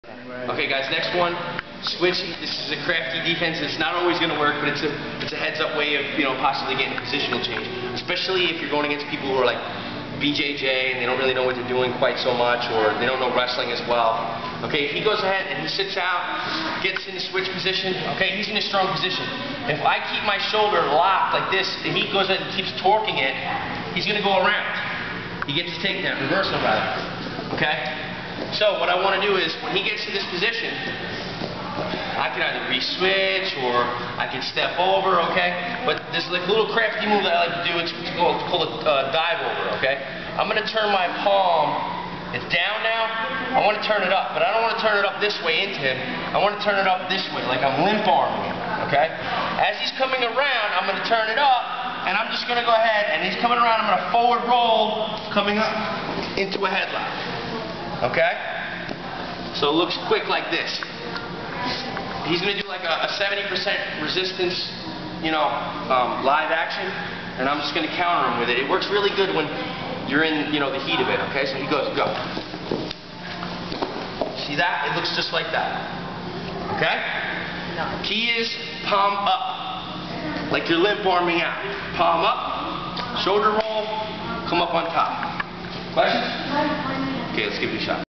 Switch Counter -Rolling Headlock-
Tricky little way to catch someone by surprise when they switch you. Probably not a move you want to rely on but something to toy around with...